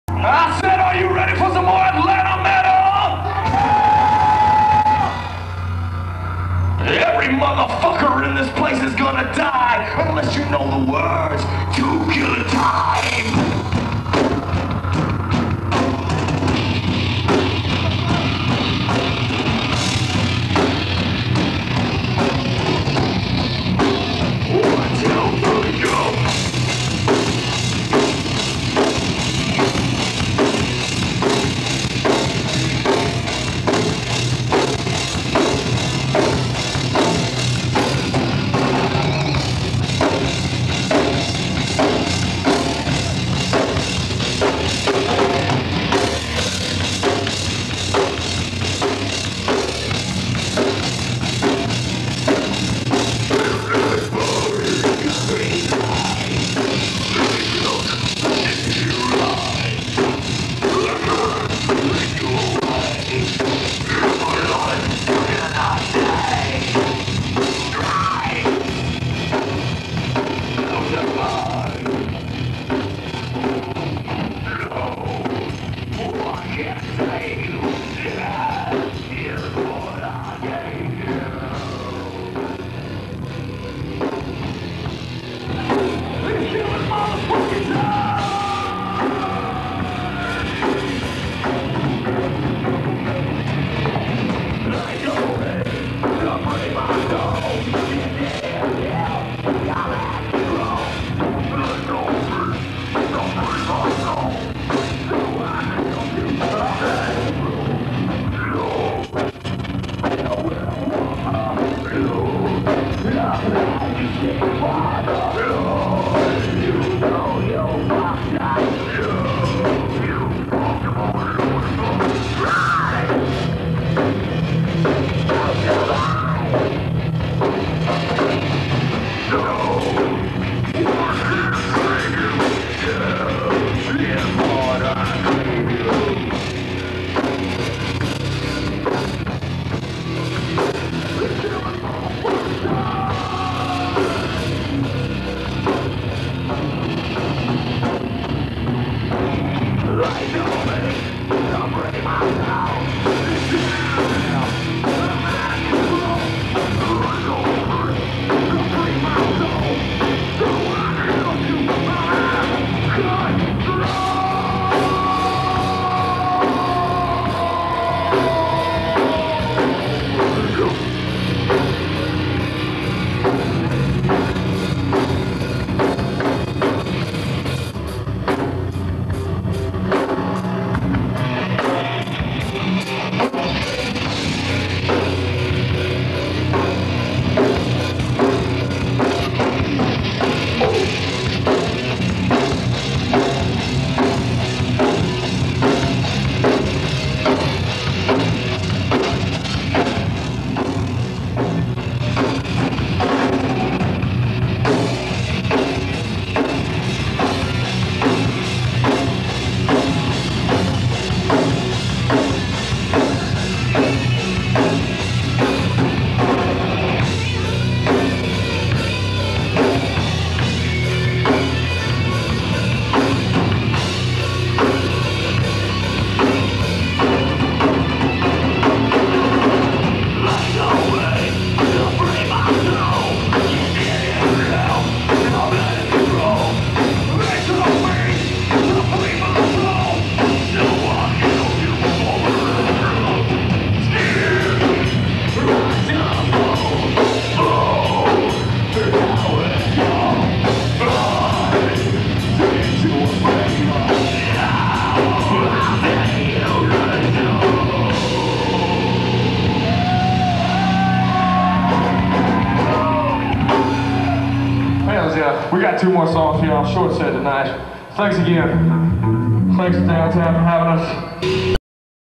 Legion X D.T.M.H. video 2
Legion X Plays  Down Town Music Hall
Sat Jan 17th
 True Southern Metal, Like a younger heaver Pantera
Light show !